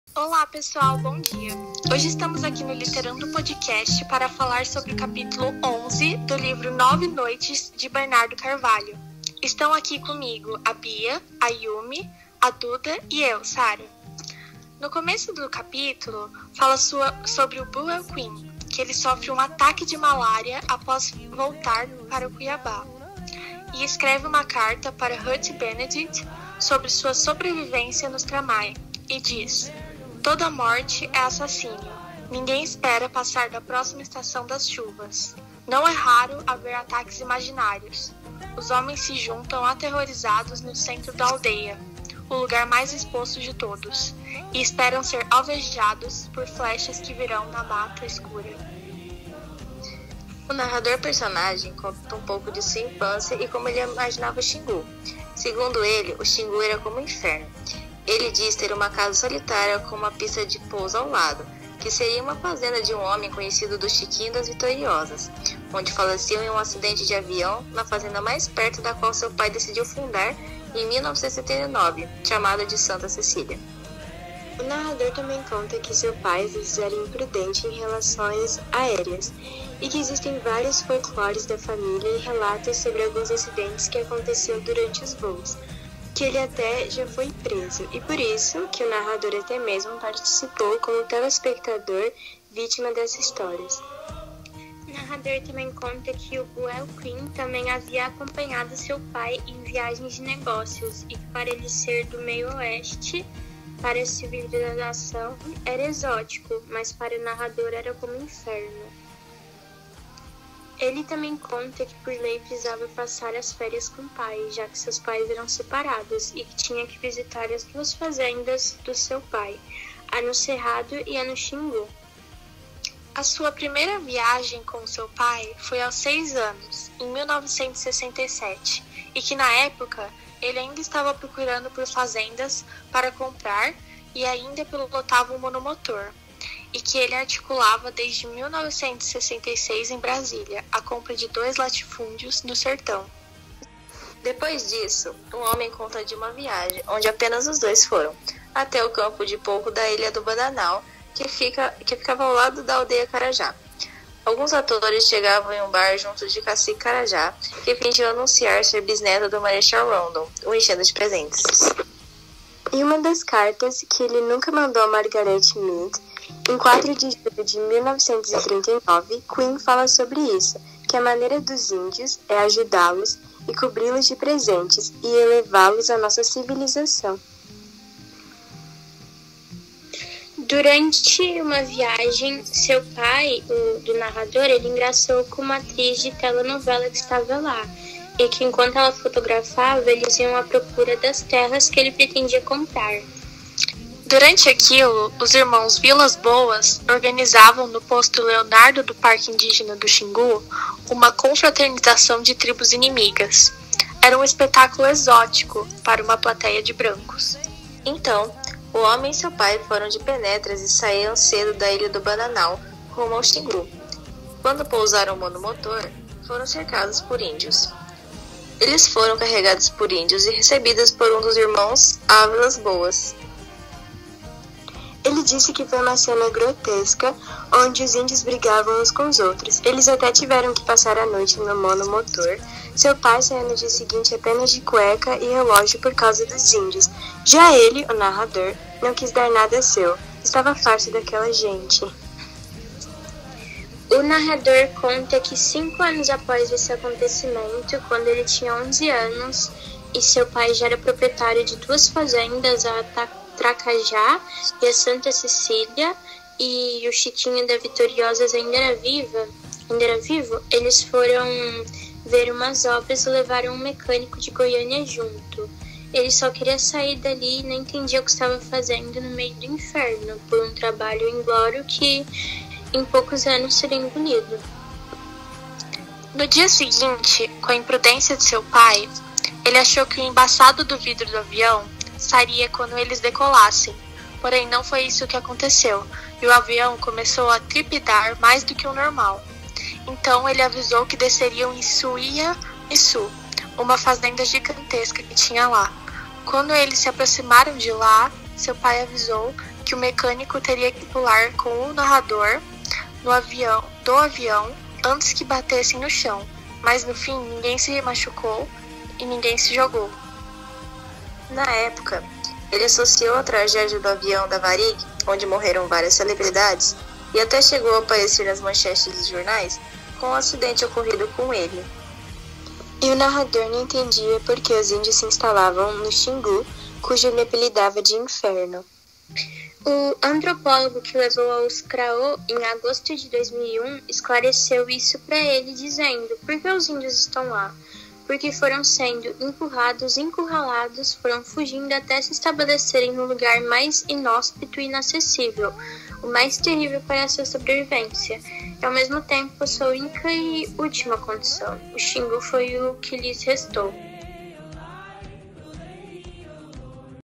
Literando Podcast Capítulo 11 (Professoras Karla e Erika) Nove Noites de Bernardo Carvalho.
Narradoras: Sarah Franco
                      Beatriz Torres
                       Yumi Sudo
                      Maria Eduarda Garcia

Editoras: Maria Eduarda de Lucca
                 Gabriely Oliveira de Pina